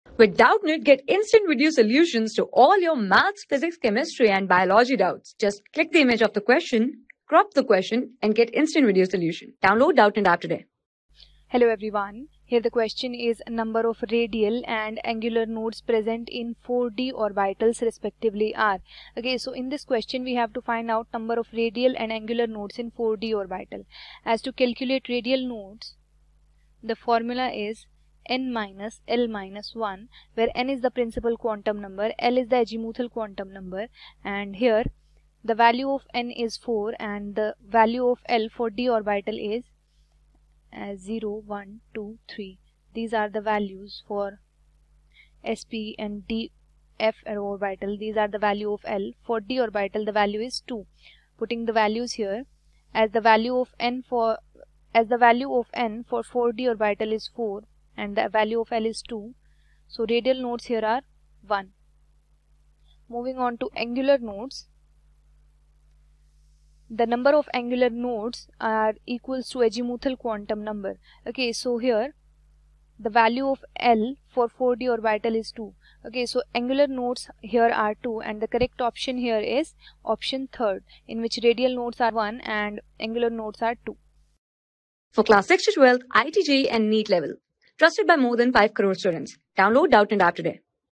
Number of radial and angular nodes present in 4d orbitals respectively are | 12 | TEST 1 | CHEMI...
Number of radial and angular nodes present in 4d orbitals respectively are
Class: 12
Subject: CHEMISTRY
Chapter: TEST 1
Board:IIT JEE

👉 Have Any Query? Ask Us.